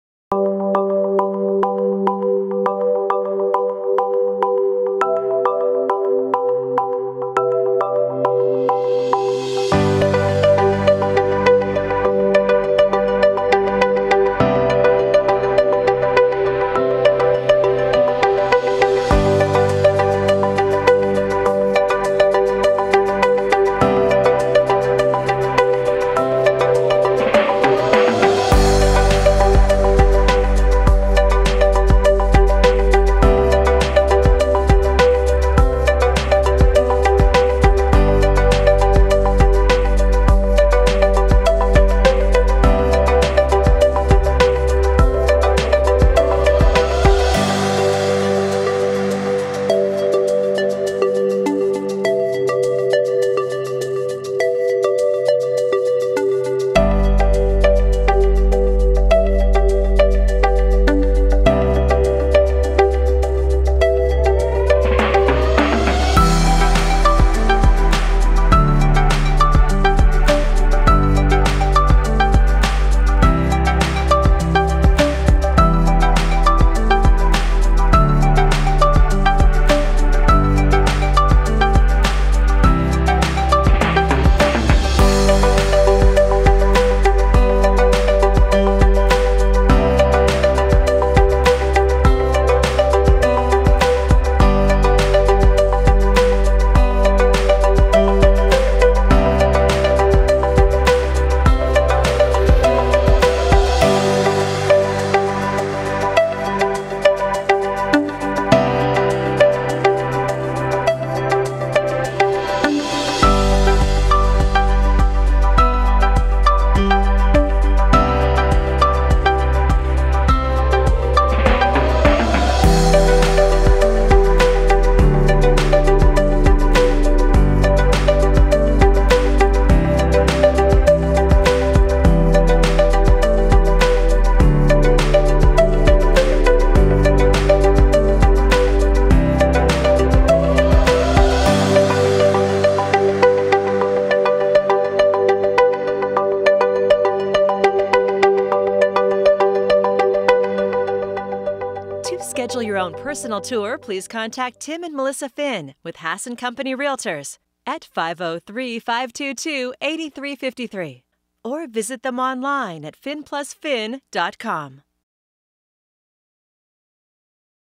Beautiful Modern Farmhouse on 4 Acres ~ NW Portland homes ~ Video of 13626 NW Old Germantown Rd.
4 acres of peace & tranquility nested 15 minutes from Downtown, 3 bedrooms with a den, large family & living room, bring all the toys, surrounded by acres of trees, Valley & Coast Range views. Restored & expanded by Neil Kelly, all permitted with seismic retrofitting. Contact Tim and Melissa Finn with Hasson Company Realtors.